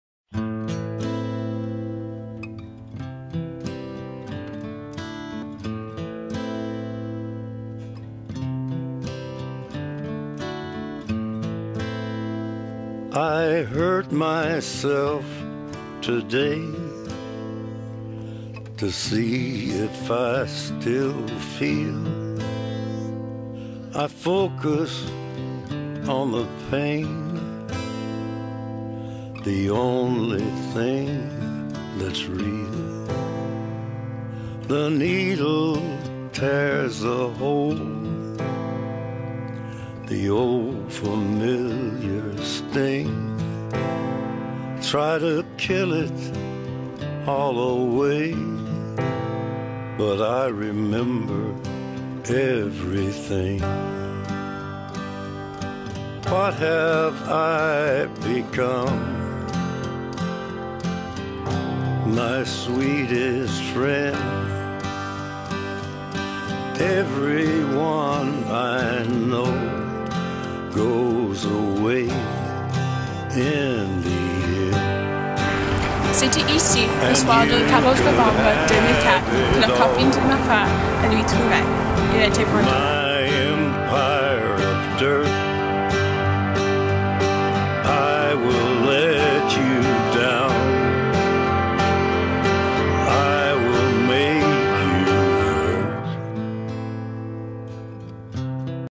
Schoo project
a french project I did with a friend on suicide in youth.